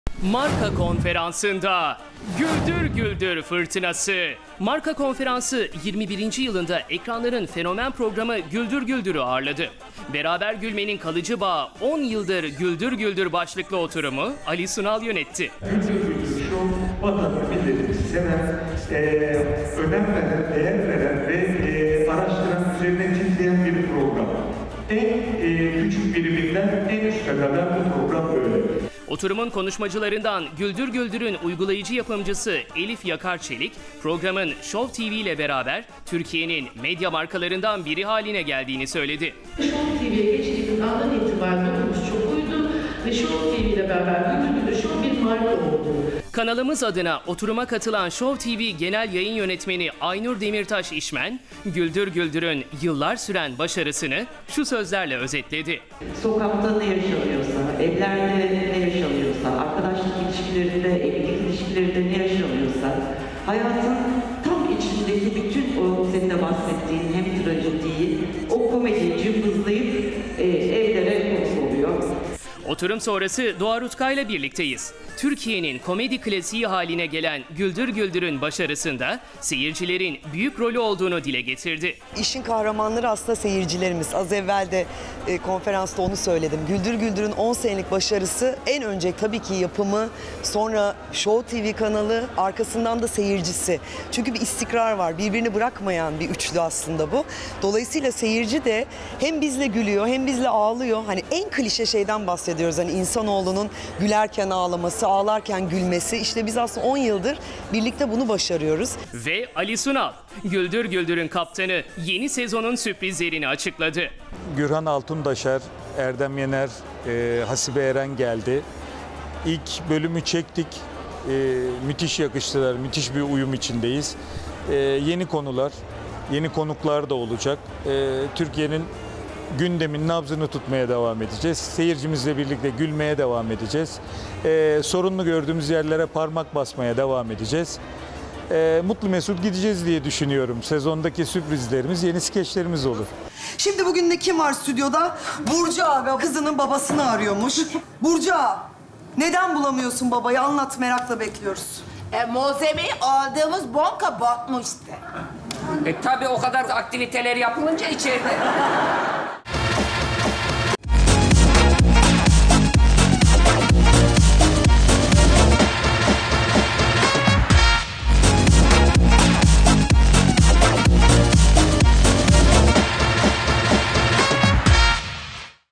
Marka Konferansı 21. yılında Güldür Güldür'ü ağırladı!
Marka Konferansı 21. yılında Güldür Güldür'ü ağırladı. Ali Sunal yeni sezonun sürprizlerini açıkladı.